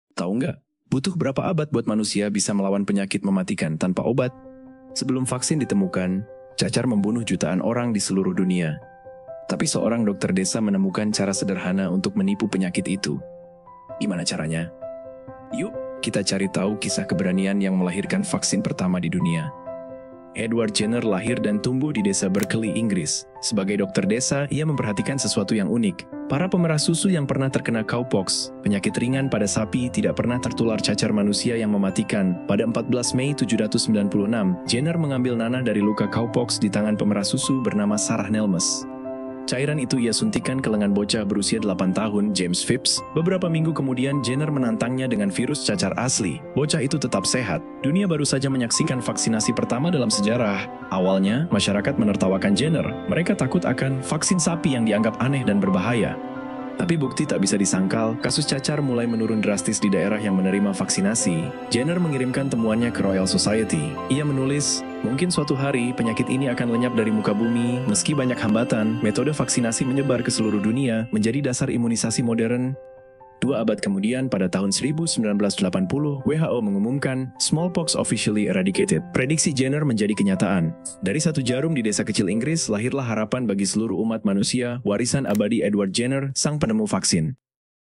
Suntikan Yang Mengubah Dunia|Edward Jenner dan Awal Vaksinasi
Edward Jenner menciptakan vaksin pertama di dunia untuk melawan cacar. 💉